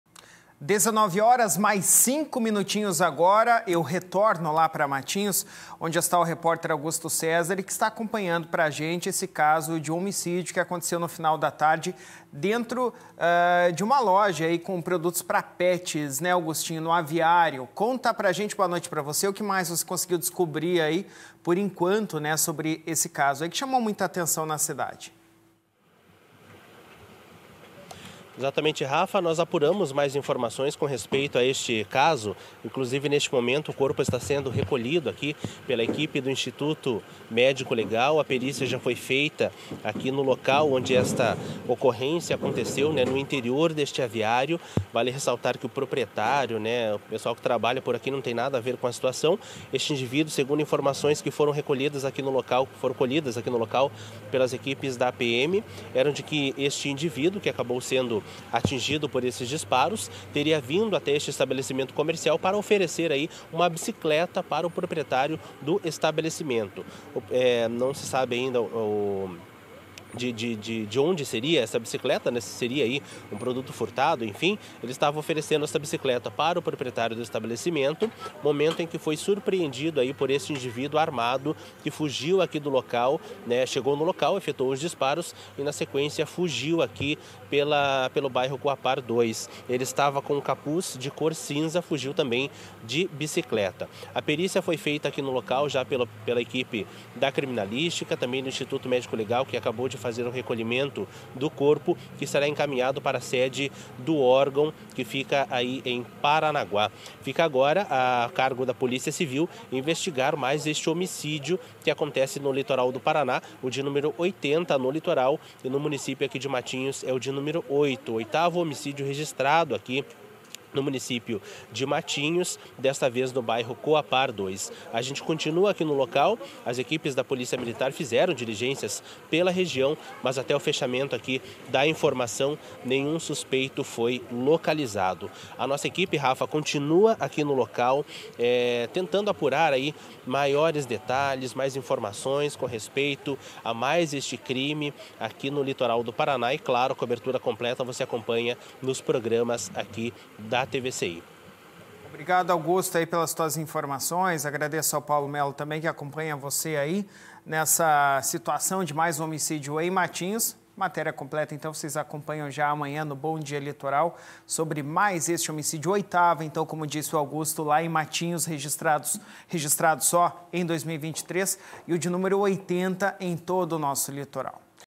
HOMEM FOI EXECUTADO EM MATINHOS:
HOMEM É MORTO, A TIROS, EM PLENA LUZ DO DIA, NO BAIRRO COHAPAR 2.